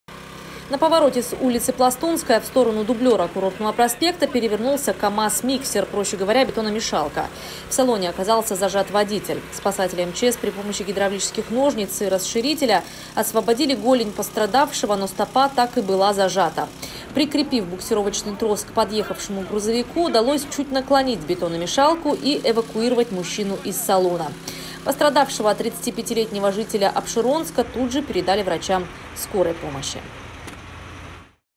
В Сочи спасатели деблокировали водителя, зажатого в салоне бетономешалки после ДТП. Новости Эфкате
Сегодня в 13.35 в Южный региональный поисково-спасательный отряд МЧС России от диспетчера службы «112» поступила информация о ДТП в районе КСМ.

Звонивший сообщил, что на повороте с улицы Пластунская в сторону Дублера Курортного проспекта перевернулся КамАЗ- миксер. В салоне зажат водитель грузового автомобиля.

Прибыв на место, спасатели при помощи гидравлического оборудования – комбинированных ножниц и расширителя – освободили голень пострадавшего, но стопа так и была зажата. Закрепив буксировочный трос за подъехавший грузовой автомобиль, удалось изменить угол наклона лежащей на боку бетономешалки, благодаря чему сотрудники МЧС смогли эвакуировать мужчину из салона машины.

Пострадавшего 35-летнего жителя Апшеронска сотрудники МЧС передали врачам "Скорой помощи", сообщает пресс-служба ЮРПСО МЧС России.